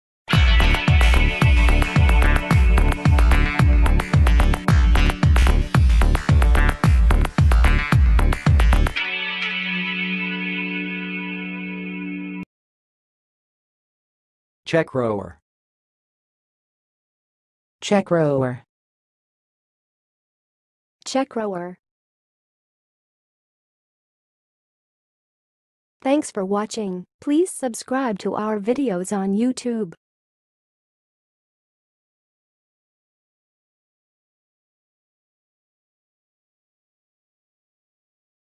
How to say "checkrower"! (High Quality Voices)
Watch in this video how to say and pronounce "checkrower"!